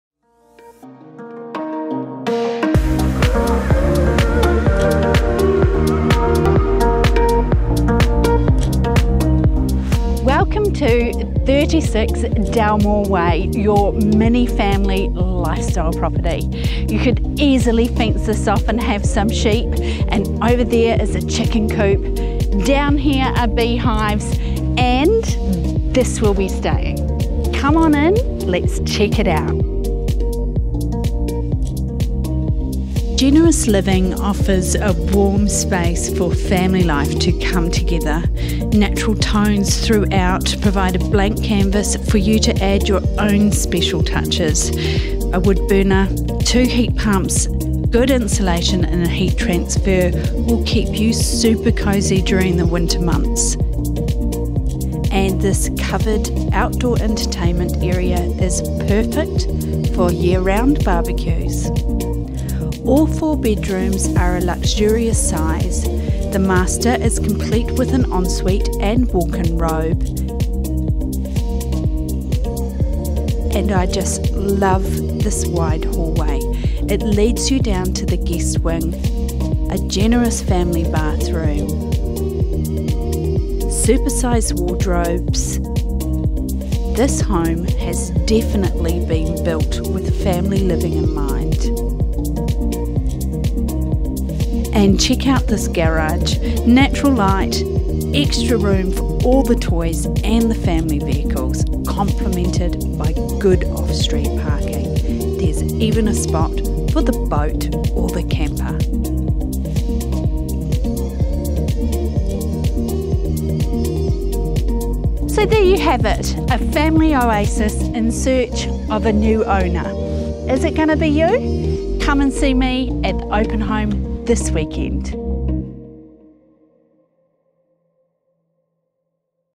36 Dalmore Way, Acacia Bay, Taupō 3385
This stunning home has everything your heart desires. A quality 2017 build, immaculately presented, spacious living, four extra-large bedrooms, sunshine, huge garaging, land - the list goes on. Sitting on a rise on 6375sqm (more or less) of land in the peaceful Dalmore Way.

The large picture windows frame the rural aspect of this property and let natural light flood in, defined living, dining and kitchen areas are open planned, spacious yet connected. Large slider doors open off the living to a covered outdoor entertainment area for year-round enjoyment, the home is complete with a wood burner, heat transfer system and two heat pumps as well as good insulation and double glazing throughout. You can be assured of toasty winter months.

The Master suite is complete with a walk-in wardrobe and en-suite, it is a spacious room with slider doors to the covered decking area, perfect for letting the summer breeze flow in. Each of the extra three bedrooms are spacious and have supersized wardrobes offering a peaceful place to retreat for each family member. Serviced by a large family bathroom, with a freestanding bathtub, polished concrete flooring and vanity.

The cherry on the top is the extra-large garaging, there's plenty of space for all the toys and a workshop. A stylish wooden playground with a climbing wall and swings will be staying as well as the chicken coop and there's even an option to fence out a paddock for a little pony or sheep. The current vendors allow for beehives at the far end of their property generating a generous amount of pure quality honey as payment.

With the bus stop and Cafe L'Arte just a short walk away, easy access to the boat ramps and top swimming spots as well as the Bay Brasserie, country living with close by amenities means you get the best of both worlds. This is a must view property so make sure you pop along to the open home this weekend.

Call Rachel for more information on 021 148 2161.

Deadline Offers on 26 July, 2022 at 3:00pm at Tremains, Taupo (unless sold prior)